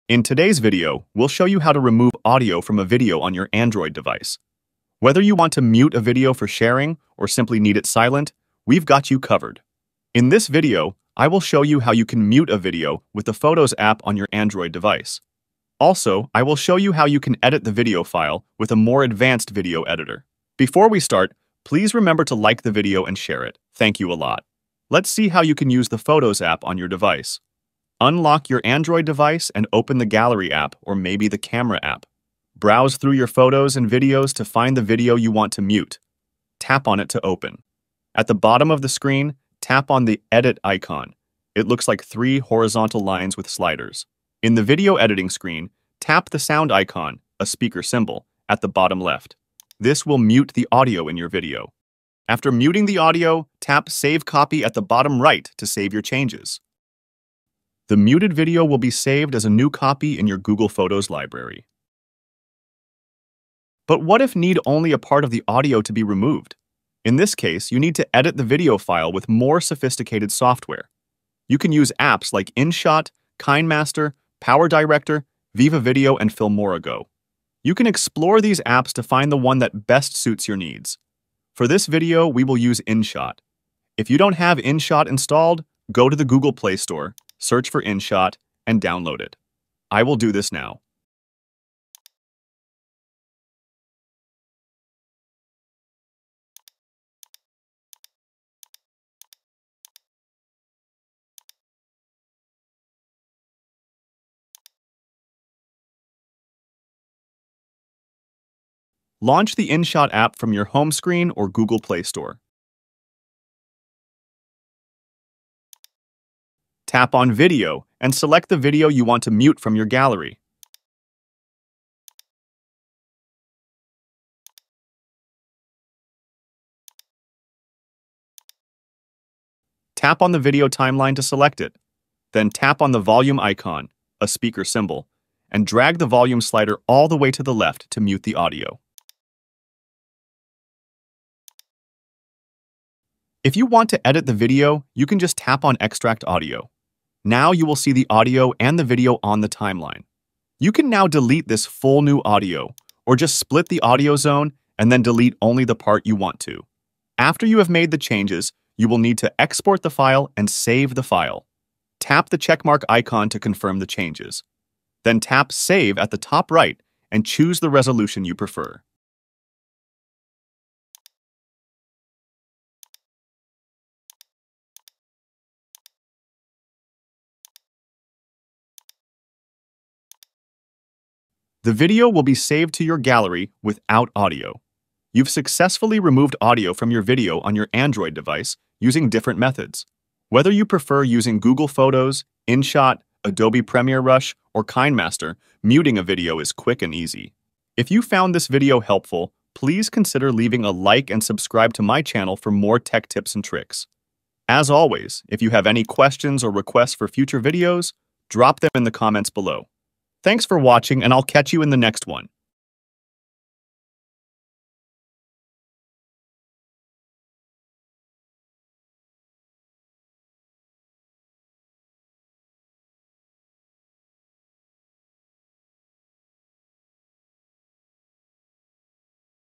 How To Remove Audio From Video on Android
Learn how to mute or remove the audio from videos on your Android device using simple methods and apps. Follow these steps to customize your videos by eliminating unwanted soundtracks.